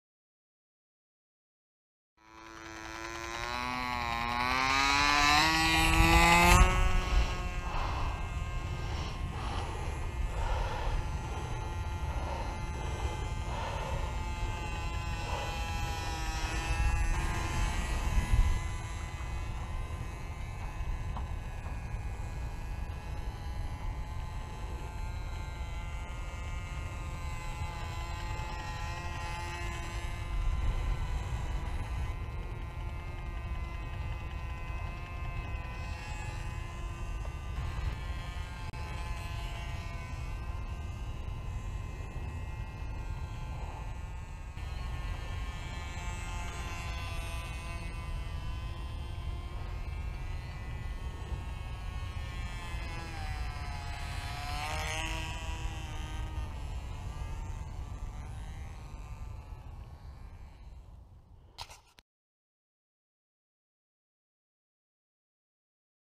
Sperry Messenger, Cox .010, 13" Span, Rudder Only
Final paint and trim has been added to my Sperry Messenger. I set the engine mixture so that the model would barely climb and flew some low circuits around the football field. It's still very hard to see such a small model in the video.